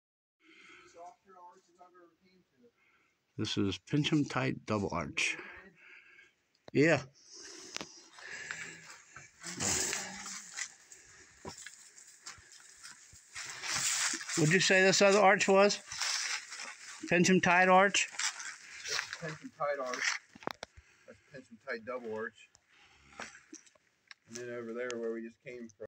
Pinch-Em Tight Double Arch and Pinch-Em Tight Arch Red River Gorge
Off of the Sheltowee Trail in the Red River Gorge Geological Area of Eastern Kentucky. It was recently found and there is no trail. Arch finder Matt Hemsath is in this video. He has found a bakers dozen verified new arches in the area as of this writing.  Visited on January 3rd, 2024.